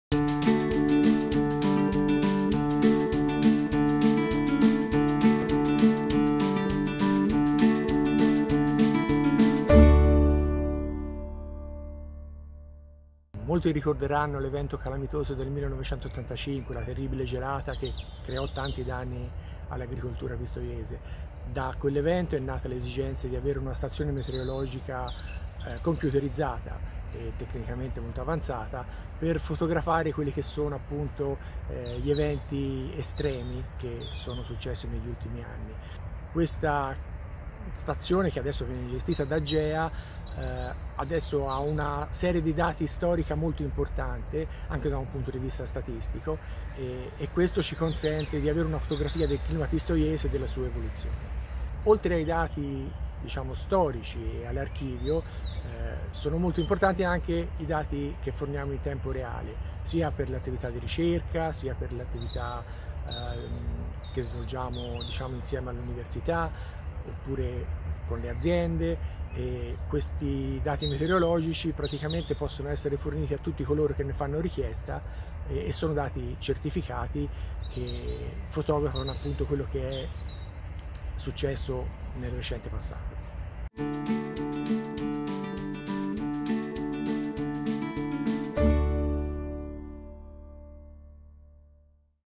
GEA - La stazione meteorologica
Dal 1989 ospitiamo una delle prime stazioni meteorologiche installate nel nostro territorio.
La stazione che oggi dispone dei più moderni ed avanzati strumenti di misura, presenta moltissimi dati di particolare importanza per la ricerca e per numerose organizzazioni e imprese del territorio.
I dati sono consultabili in tempo reale sul nostro sito web ed è, inoltre, possibile accedere all’archivio storico delle registrazioni fino al 1950. Grazie ad una collaborazione con il CNR, infatti, sono stati ricostruiti i dati dal 1950 fino al 1989, anno di installazione della stazione meteo. Questo permette di visionare la serie completa di oltre 70 anni di dati meteorologici.
Consulta i dati e le previsioni meteo in tempo reale o accedi all’archivio dati meteo